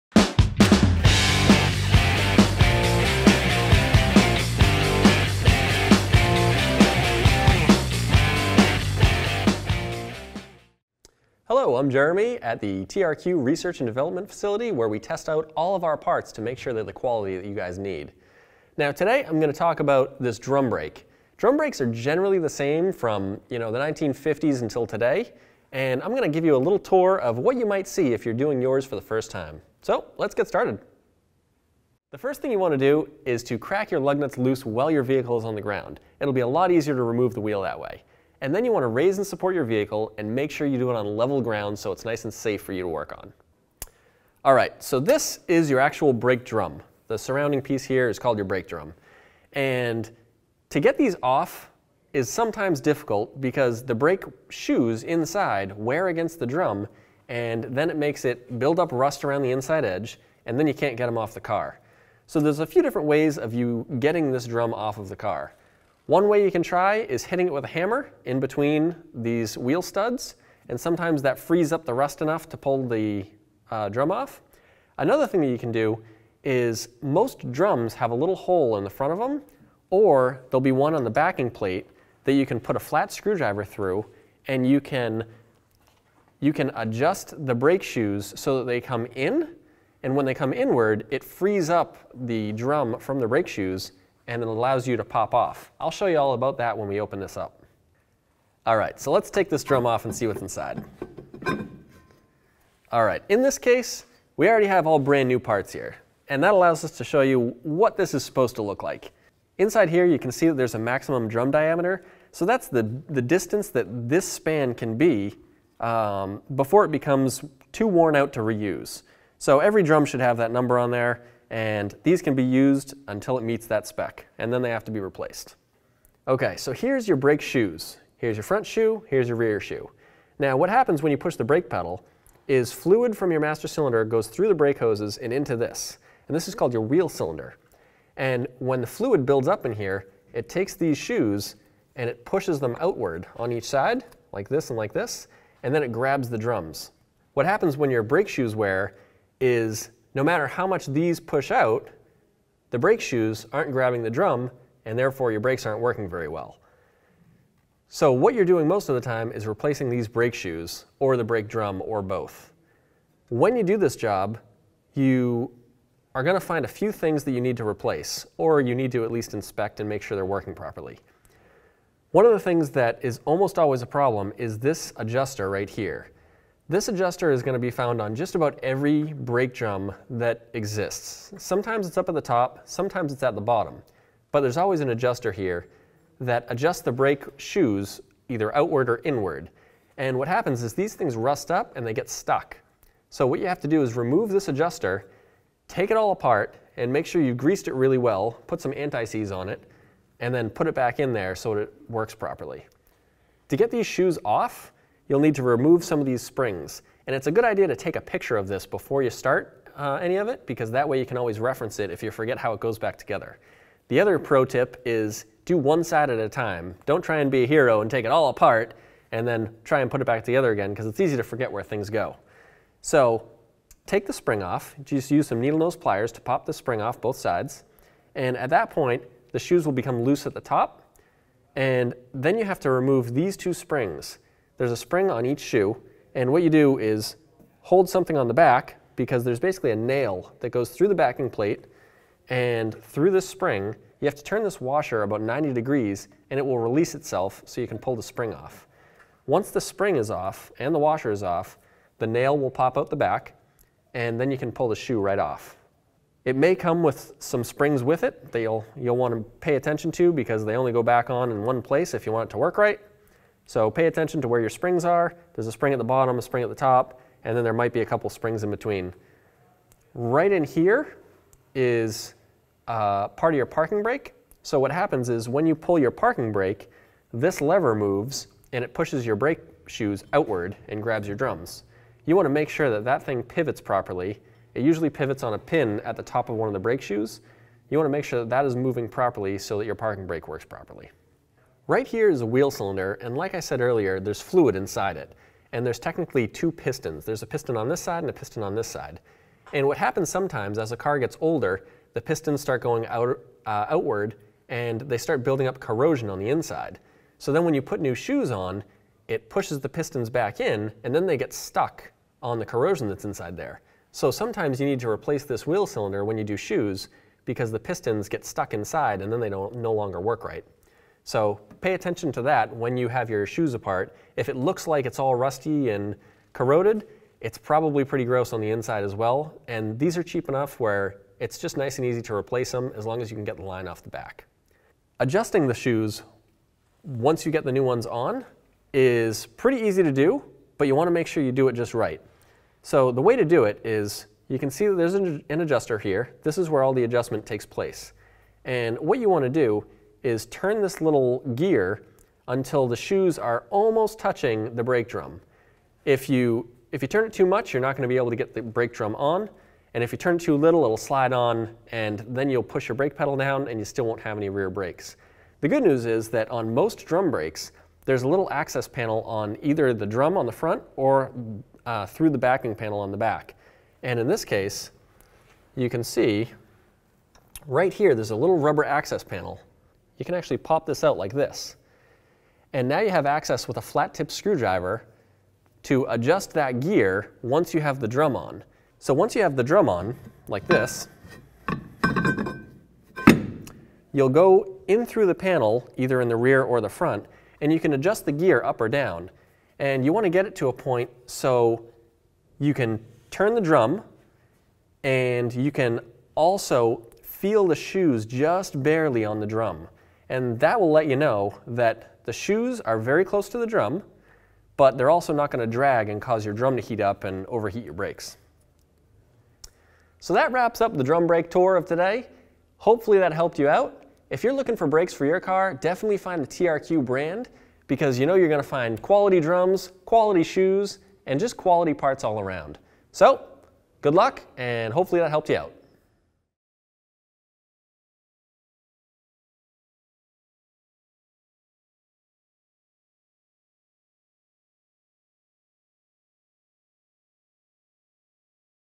How to Replace Drum Brakes on Any Vehicle
This video shows you the common features of drum brakes so that you can learn how to replace the brakes on your specific vehicle.